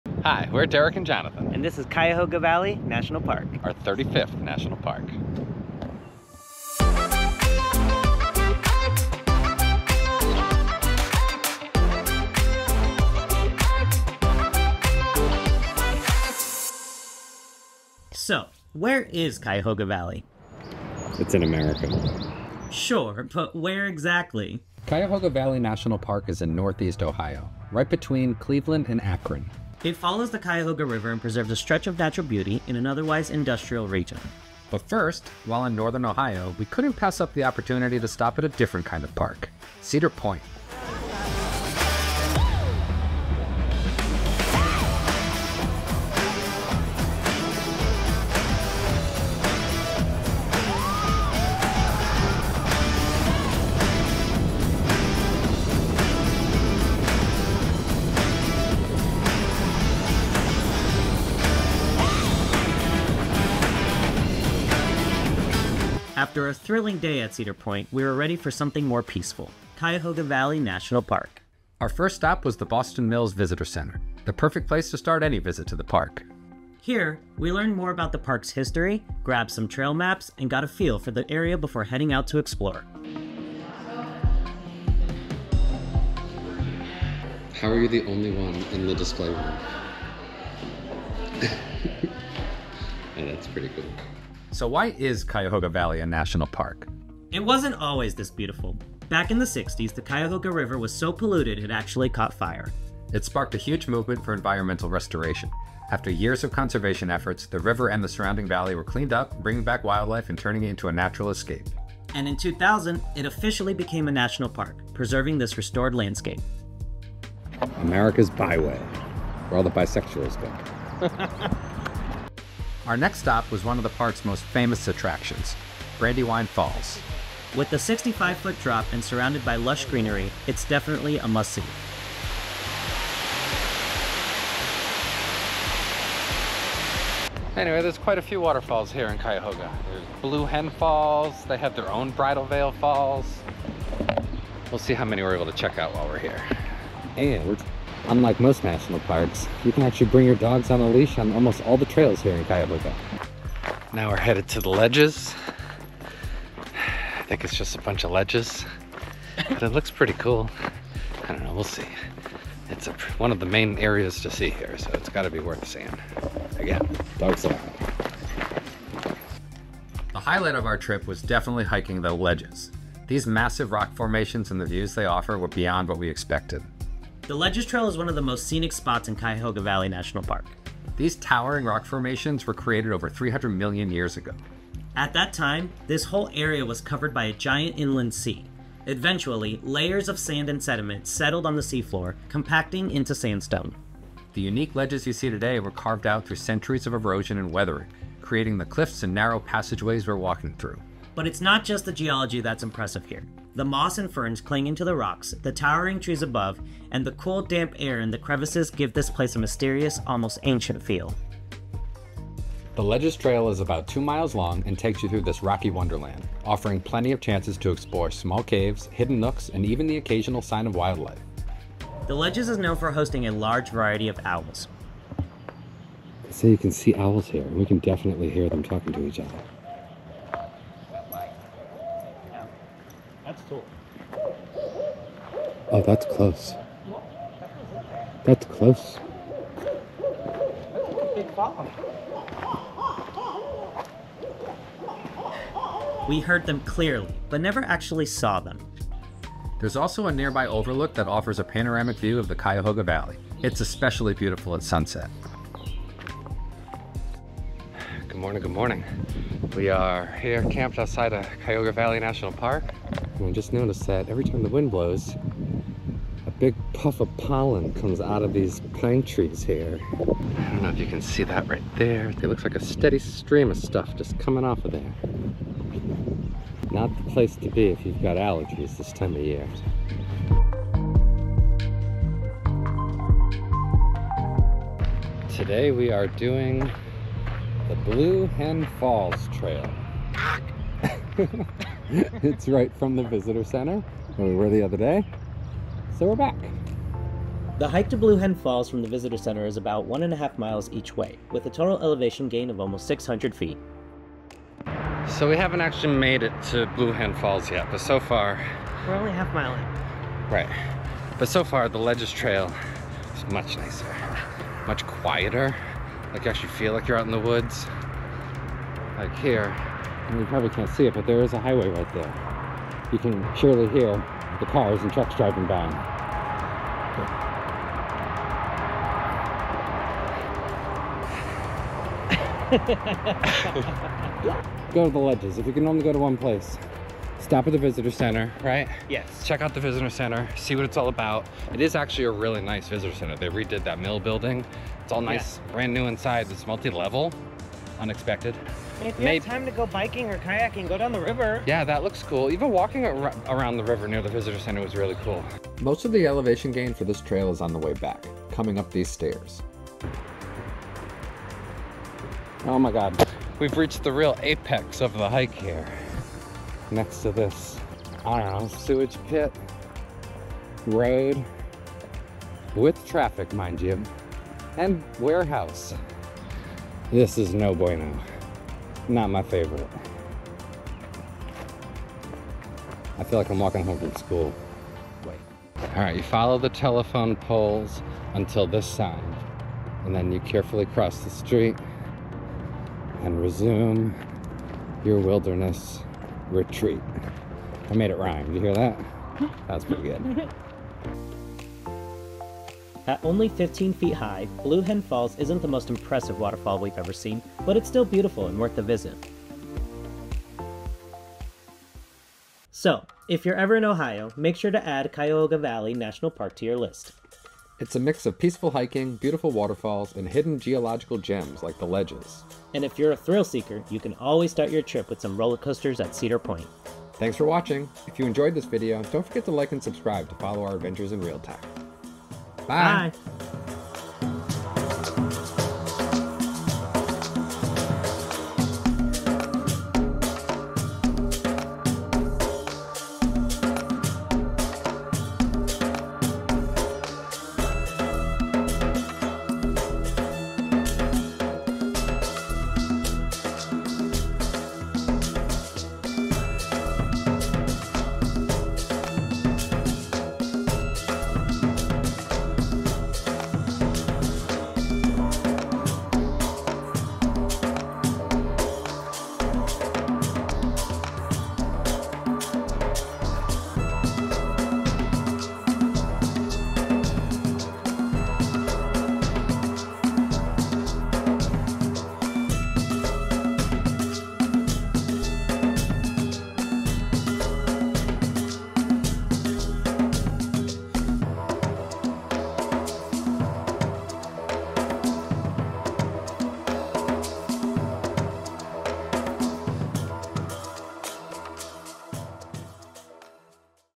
Ohio’s Ultimate Adventure: Cuyahoga Valley National Park & Cedar Point Thrills!
🌿 Ohio’s Ultimate Adventure awaits! In this video, we explore Cuyahoga Valley National Park and get our adrenaline pumping at Cedar Point Amusement Park! 🎢

We start with a day of thrills at Cedar Point, famous for its record-breaking roller coasters and breathtaking sunset views 🌅. After all the excitement, we dive into the natural beauty of Cuyahoga Valley National Park, an underrated gem in Ohio.

Our adventure begins at the Boston Mill Visitor Center before heading to the iconic Brandywine Falls, one of the park’s most stunning waterfalls. Next, we hike the picturesque Ledges Trail, where the breathtaking rock formations and forest views truly steal the show 🏞️.

On day two, we check out Blue Hen Falls—a smaller but charming waterfall that offers a peaceful retreat into nature. Whether you're planning a visit or just exploring from home, this video takes you through some of the best hikes, waterfalls, and sights that Cuyahoga Valley National Park has to offer!

🛑 Key Highlights:

    Brandywine Falls: The must-see waterfall of Ohio 💧
    The Ledges Trail: Awe-inspiring rock formations 🪨
    Blue Hen Falls: A serene nature escape
    Cedar Point: Tips for the ultimate amusement park adventure 🎢

Hit play to join us on this incredible Ohio adventure! 💬